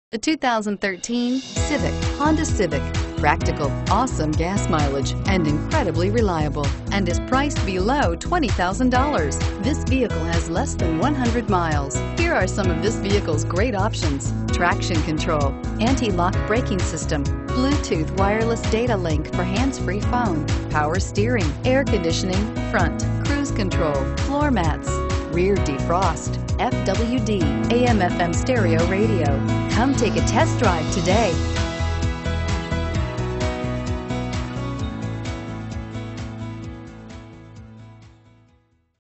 2013 Honda Civic Cpe Irvine Orange County CV3885
Fladeboe Honda
16 Auto Center Dr

Looking for the right car? Today could be your lucky day. With 4 miles, this NH-578/TAFFETA WHI, 2013 Honda Civic Cpe equipped with Automatic transmission could be yours. Request more information and set up a test drive right away.